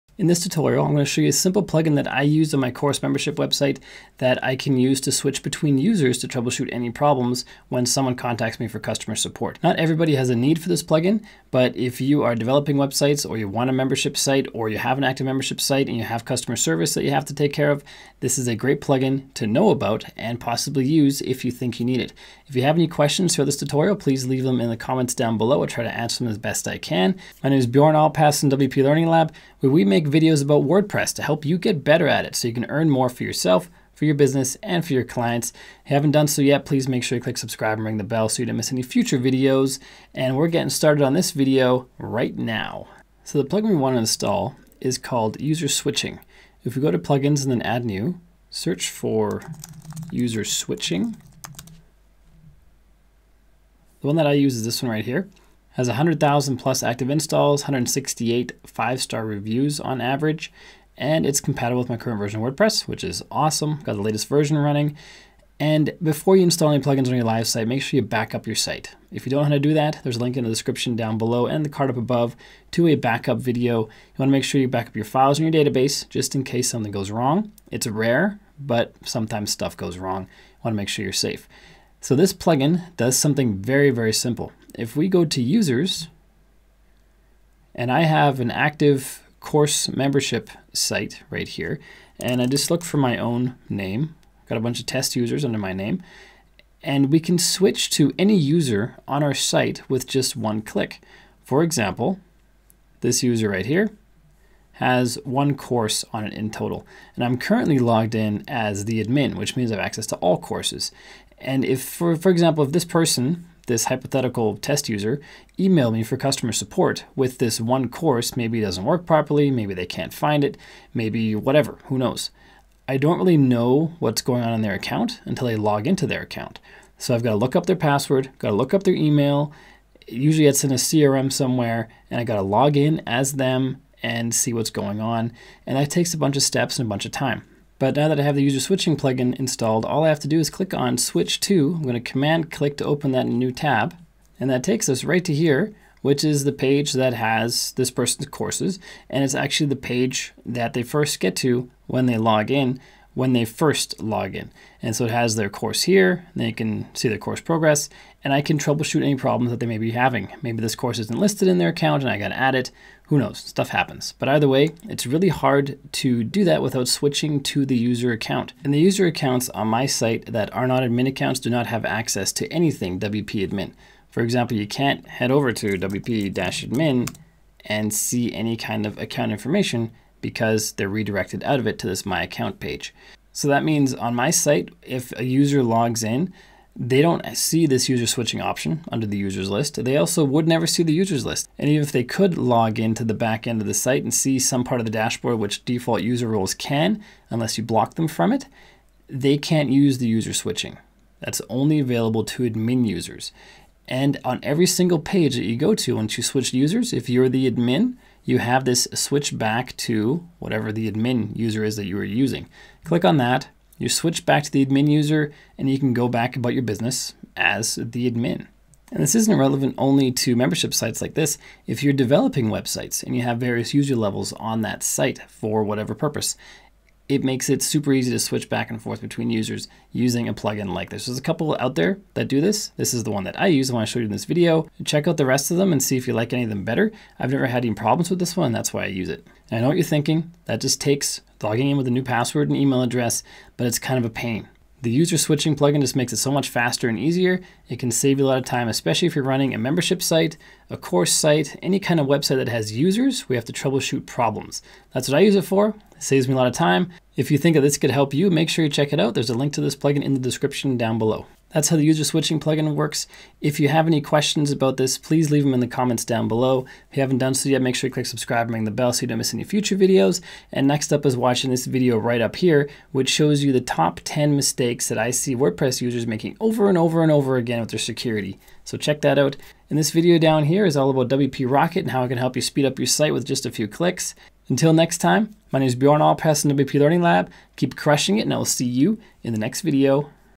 How This User Switching Plugin Can Save Your Loads Of Time
What is user switching?

It is when you, as the admin, can switch to be logged in as any other user on your site with just one click.

No need to log out and log in with a new username and password.

This can save you lots of time with testing during development and customer support for membership or course sales websites.